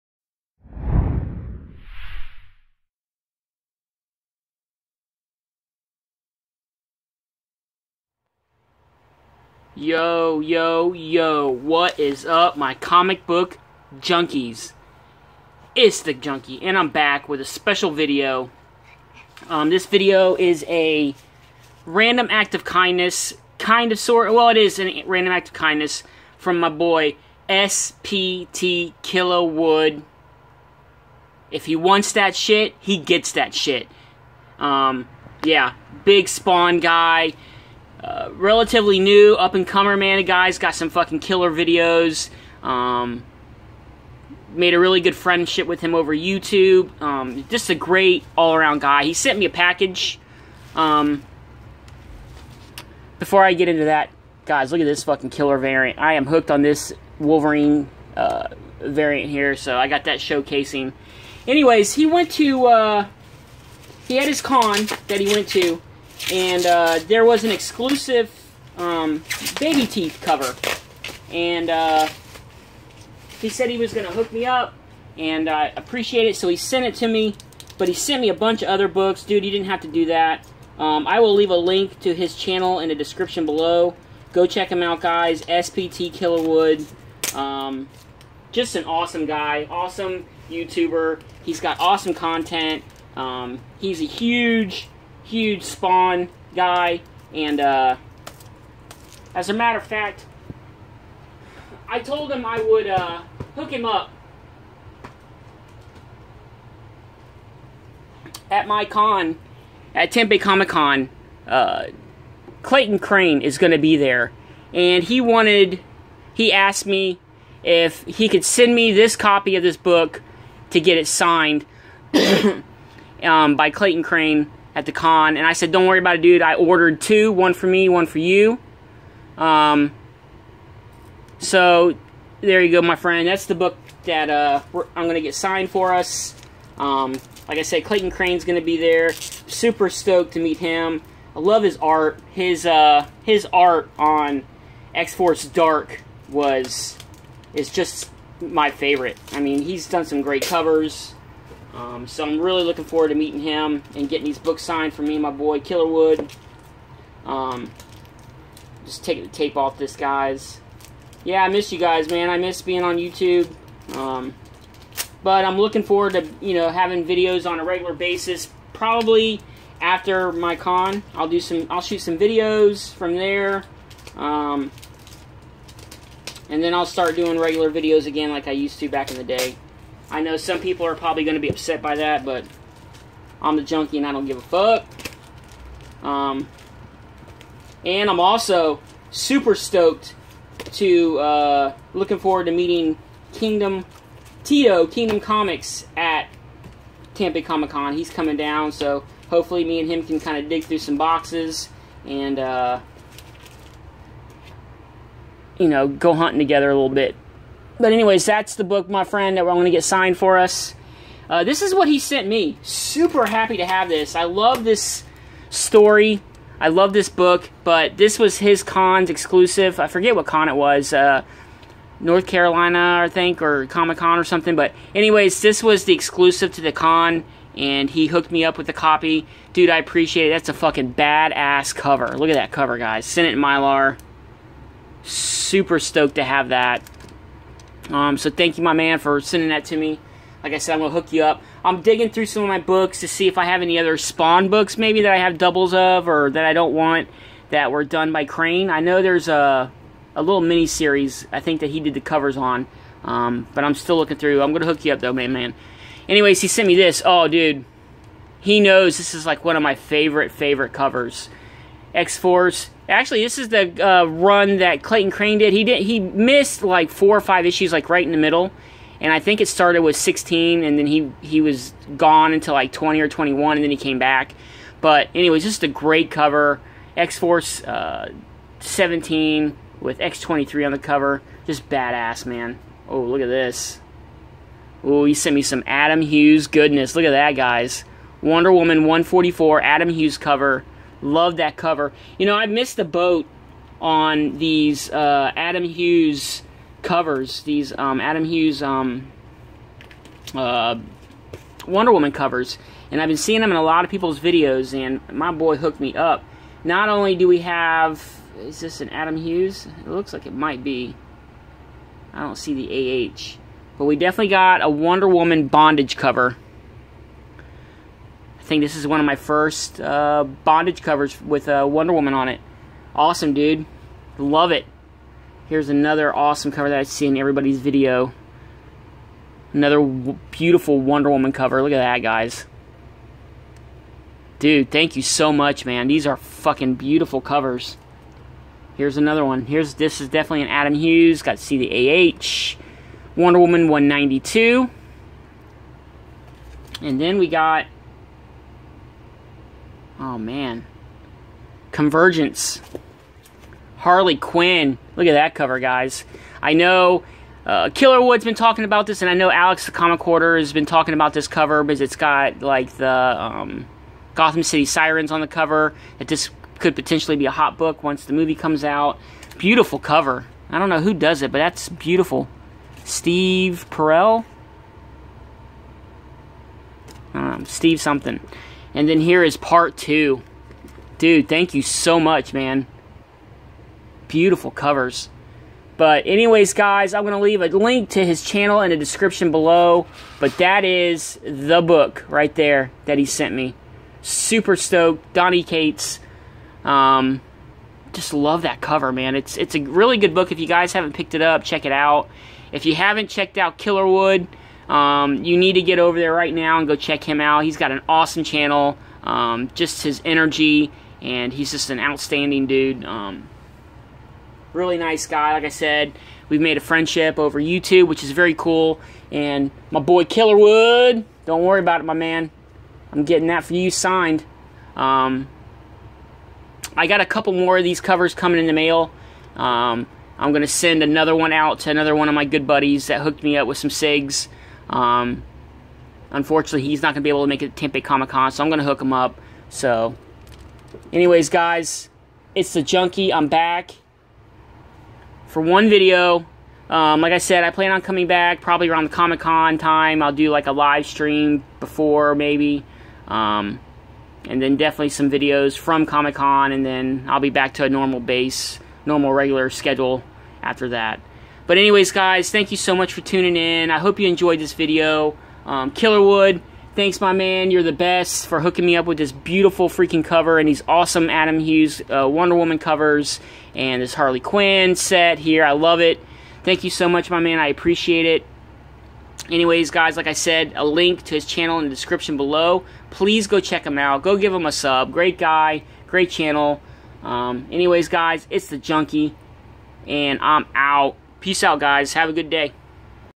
AAOK FROM SPT KILLERWOOD: BABYTEETH AND ADAM HUGHES !!
IN THIS VIDEO: AAOK FROM SPT KILLERWOOD: BABYTEETH AND ADAM HUGHES !! MY BOY KILLERWOOD HOOKED ME UP!!